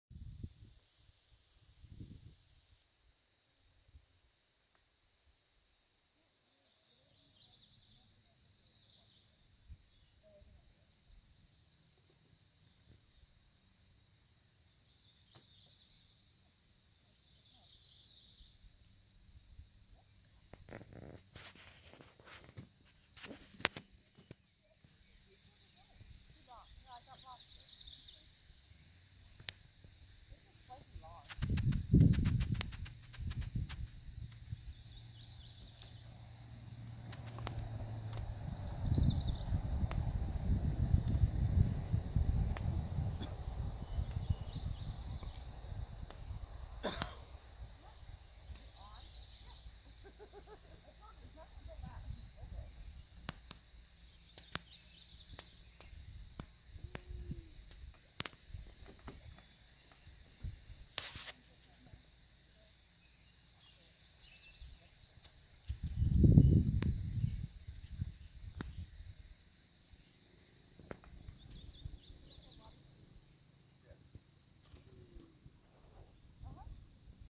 20200712 Perry Sound Cottage Life 08
Enjoying a weekend up at a cottage near Parry Sound Ontario.
Nice having one's own private pond.